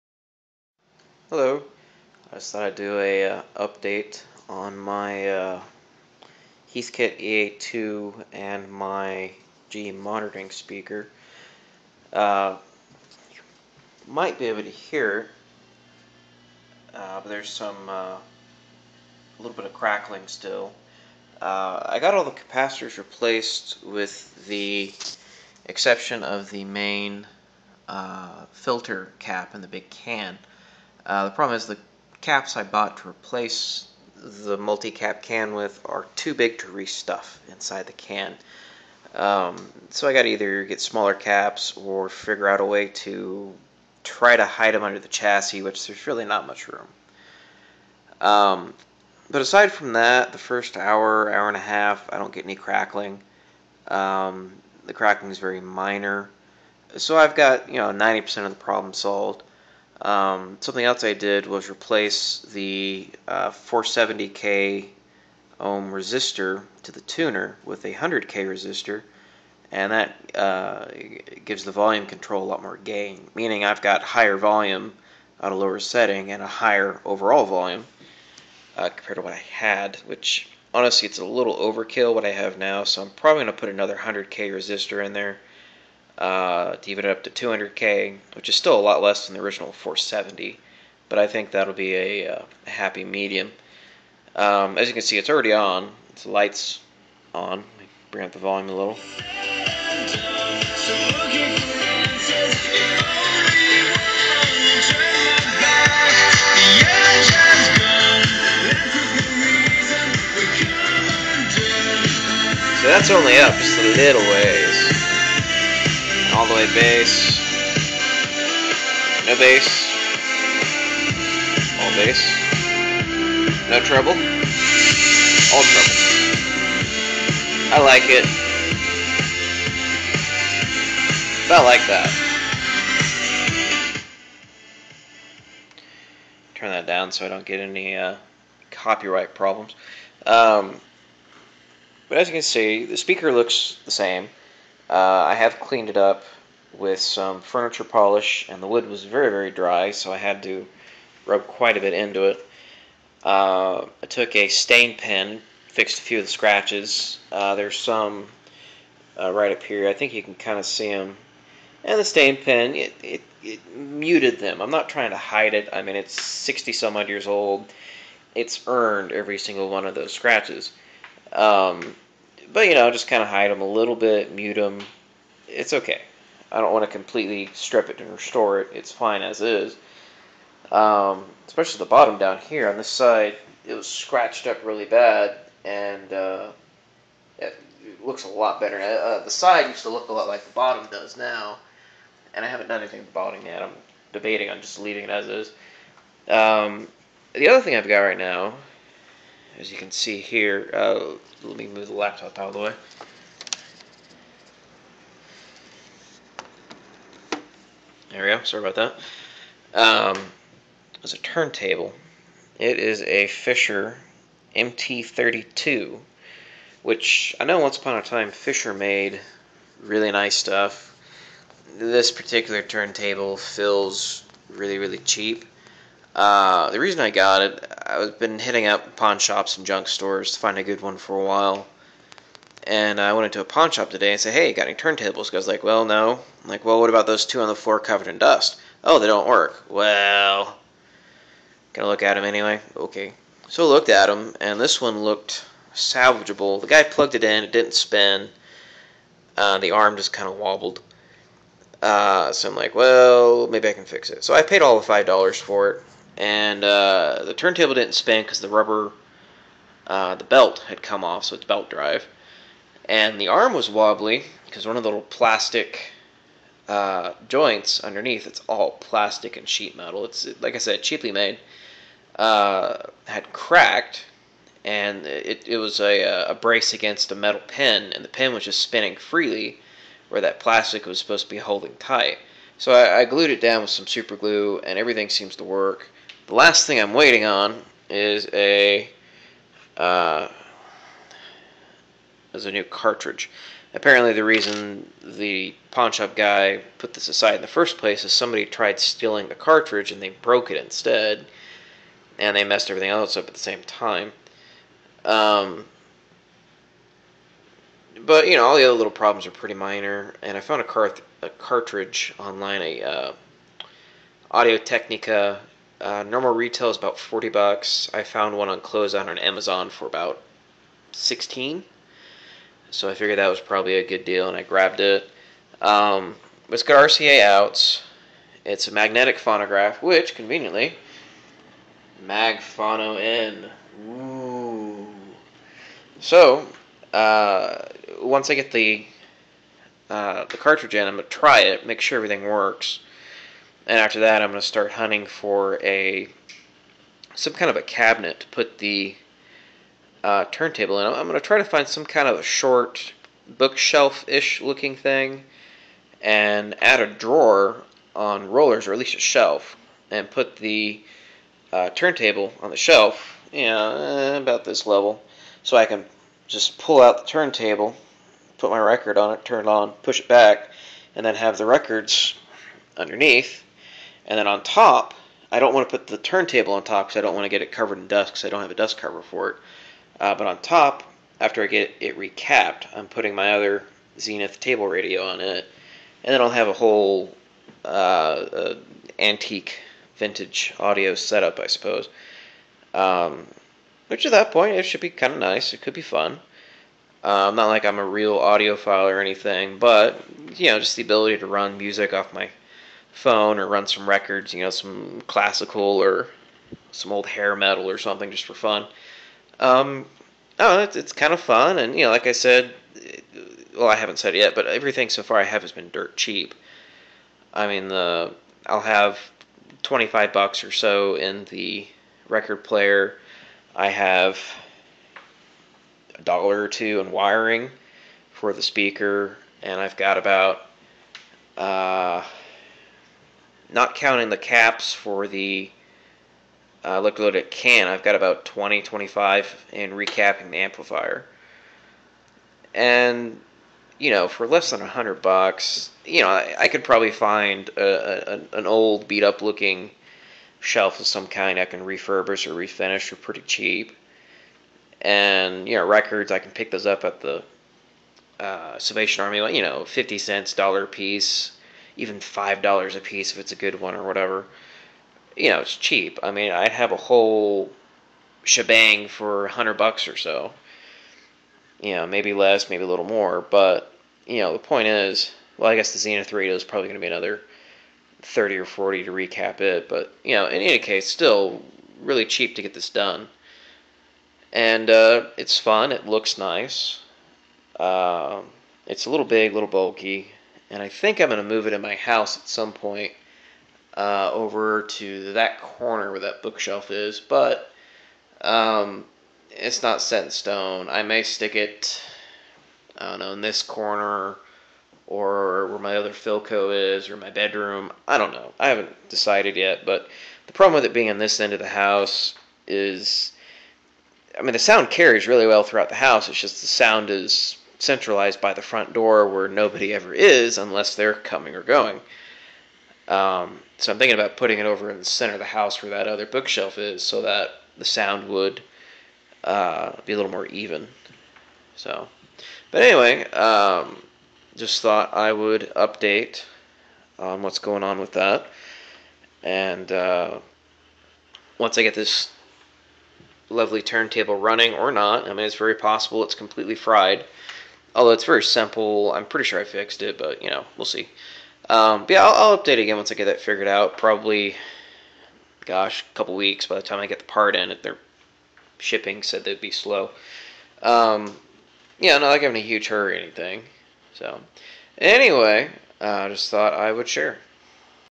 Post re-cap Heathkit EA-2 Amplifier test and current setup
Edit:  I have the Fisher MT 32 working now!

I think I have the worst of the gremlins chased out of the EA-2.  I also did a little work to the GE Monitoring Speaker to elminate a hum by adding some staples to a mesh grill inside and adding some foam insulation to the back hatch to keep it from rattling.

The Heathkit just needs another resistor added to the tuner input and the electrolytic filter can caps replaced, but that project is currently in the research stage.  The replacement caps I have on hand are too large to fit inside the existing can if I gutted it so I'm looking into alternatives.

I also picked up a turntable that will hopefully work after I get my replacement needle/cartridge in but I only paid $5 for it so if it doesn't work I won't be devasted.

Otherwise, everything seems to be working out allright.